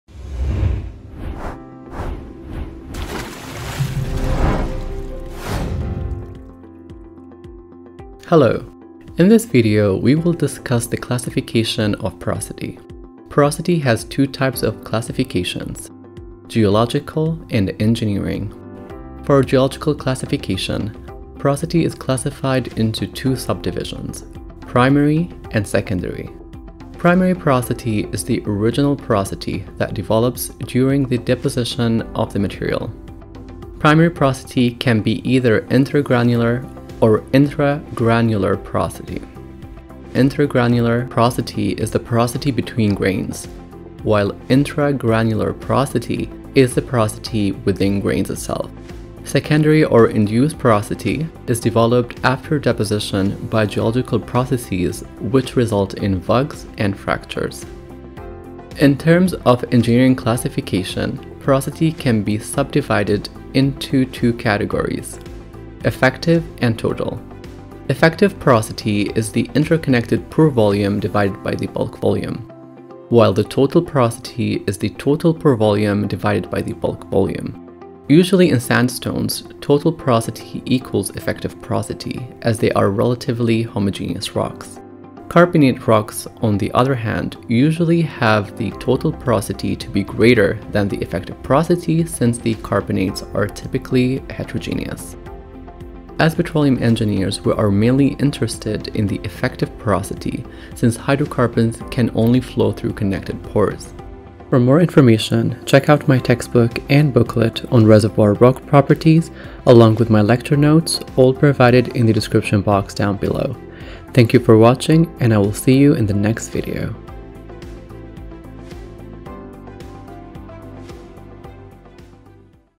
Classification of Porosity
Classification to Porosity:
00:00 Porosity Classifications
00:21 Geological Classification of Porosity
00:58 Engineering Classification of Porosity